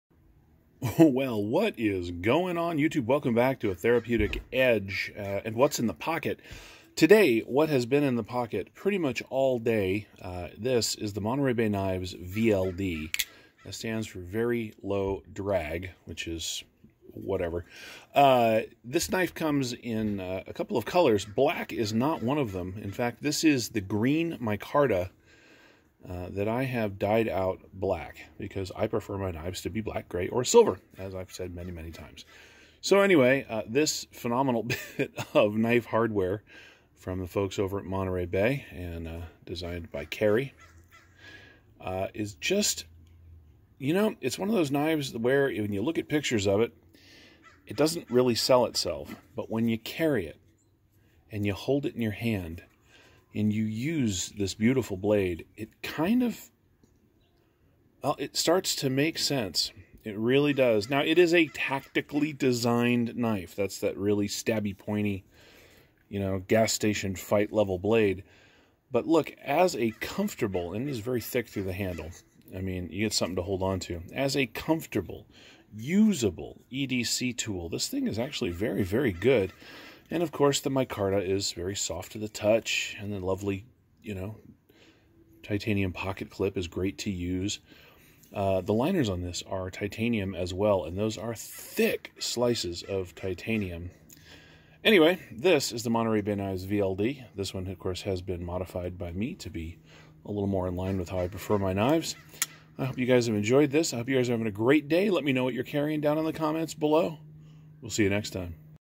What's in the pocket? (11/29/20)
In the pocket today is my slightly modified MBK VLD

What's in your pocket today?